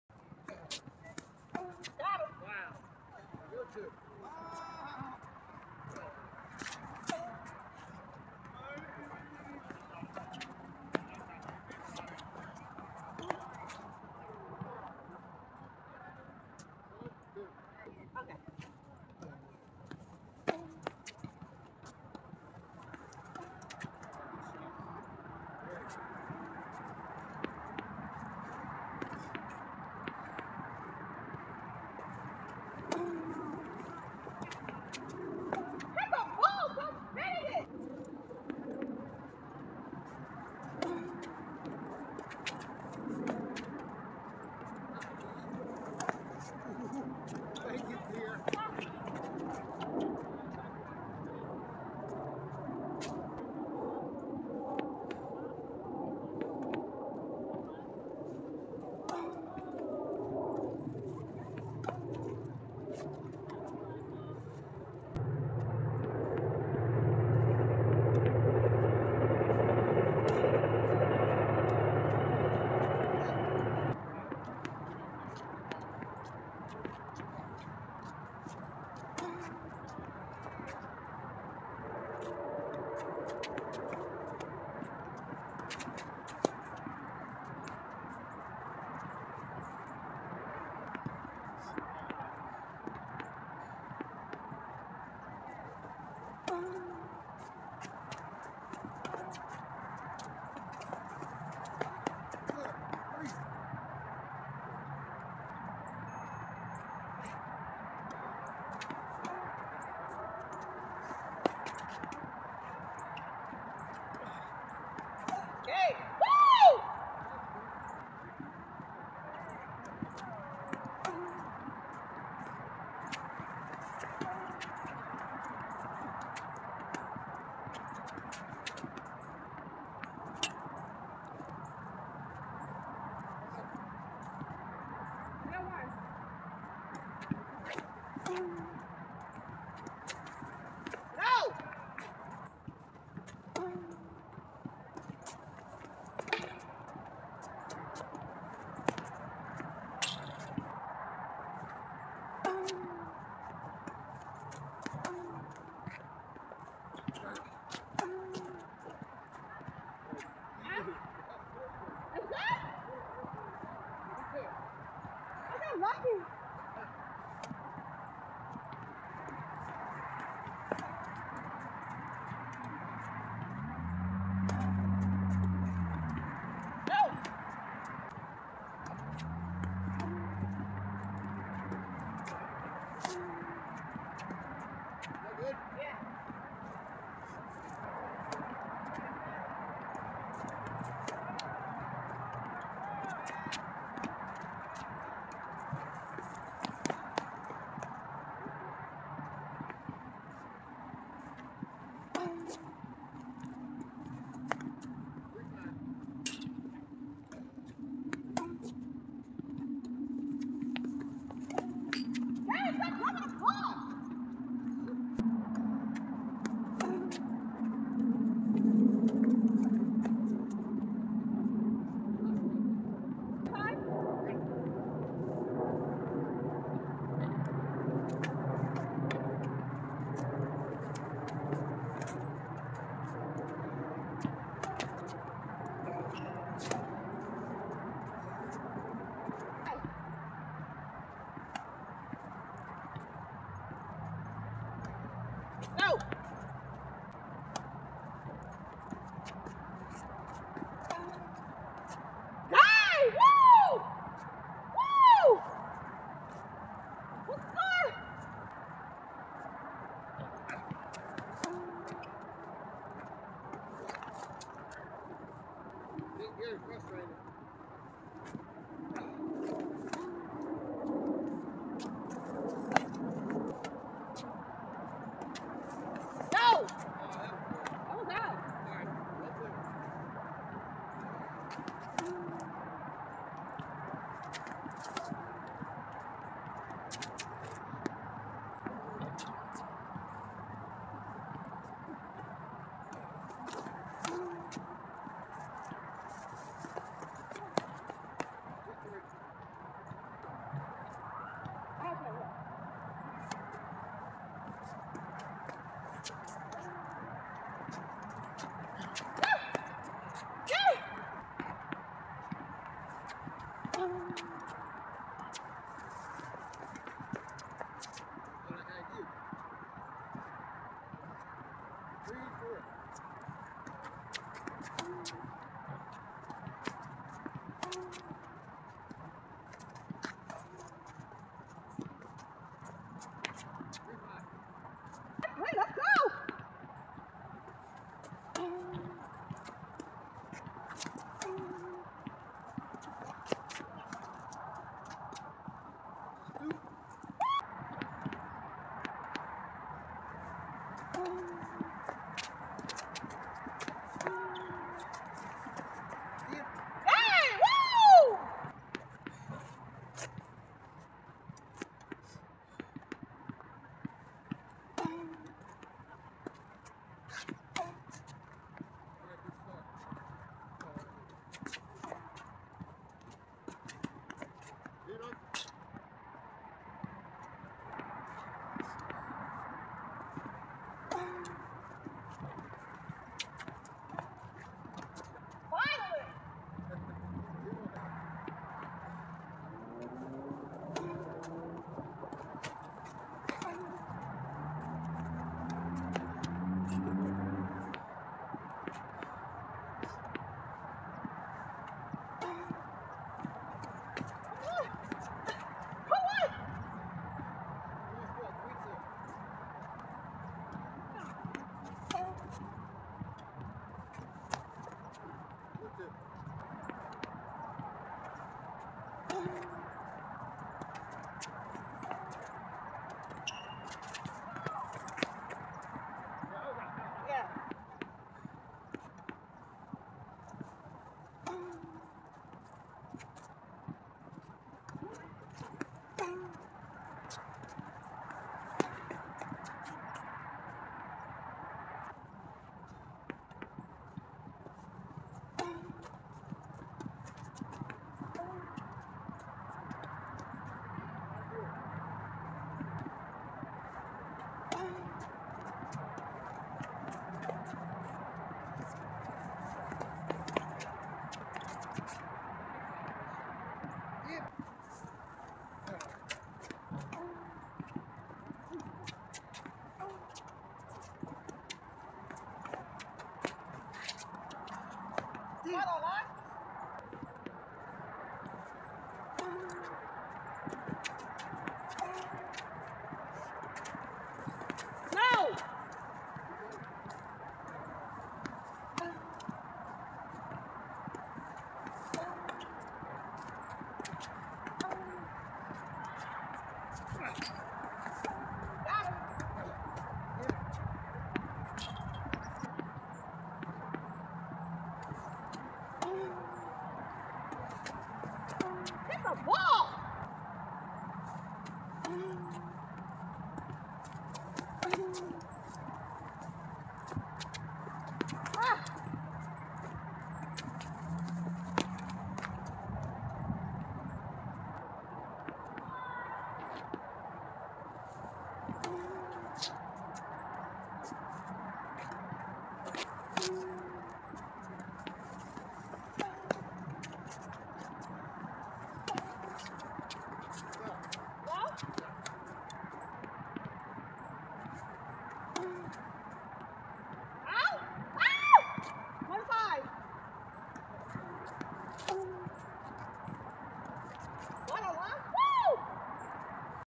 groundhog day tennis drills 🥶🎾 2.2.23
tennis tennisdrills february cold on this cold February 2, 2023 groundhog day. Gary and Austin do tennis drills.